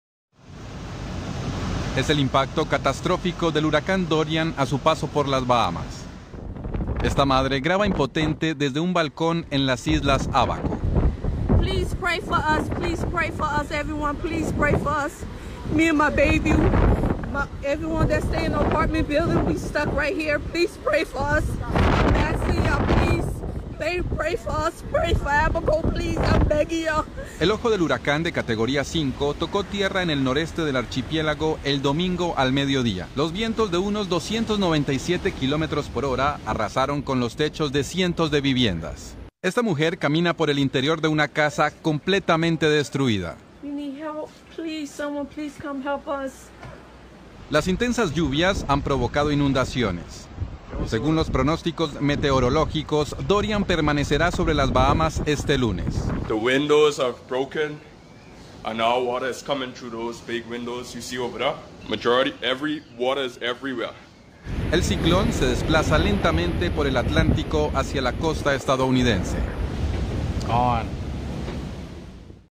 Catastrófico paso del huracán Dorian por las Bahamas.
El ojo del huracánDorian, de categoría 5, tocó tierra en las Bahamas el domingo por la mañana.
Residentes de las IslasÁbaco documentaron toda la fuerza del meteoro.

Una mujer suplica en una transmisión que recen por ella ya que estaba atrapada en un edificio junto a su hijo.